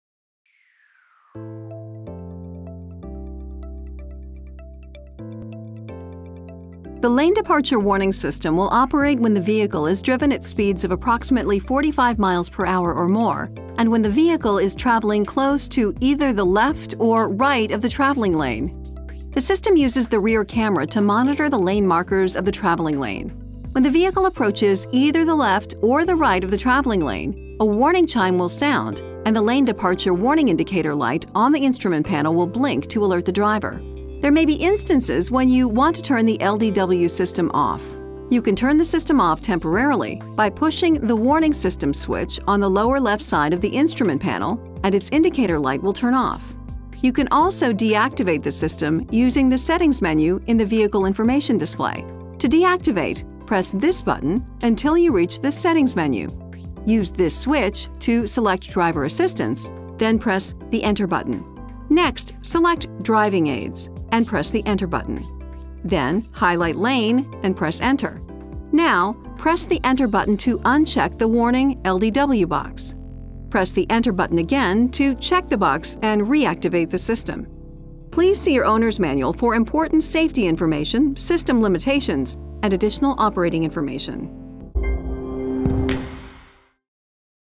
2015 Nissan Altima - Lane Departure Warning (LDW) (if so equipped)
"The Lane Departure Warning System will operate when the vehicle is driven at speeds of approximately 45 miles per hour or more and when the vehicle is traveling close to either the left or right of the traveling lane.

The system uses the rear camera to monitor the lane markers of the traveling lane. When the vehicle approaches either the left or the right of the travelling lane, a warning chime will sound and the Lane Departure Warning indicator light on the instrument panel will blink to alert the driver.

There may be instances when you want to turn the LDW system OFF. You can turn the system OFF temporarily, by pushing the WARNING SYSTEMS switch on the lower left side of the instrument panel and its indicator light will turn OFF. You can also deactivate the system using the "Settings" menu in the Vehicle Information Display. To deactivate, press THIS button until you reach the "Settings" menu. Use THIS switch to select "Driver Assistance" then press the ENTER button. Next, select "Driving Aids" and press the ENTER button, then highlight "Lane" and press ENTER. Now, press the ENTER button to uncheck the "Warning (LDW)" box. Press the ENTER button again to check the box and reactivate the system.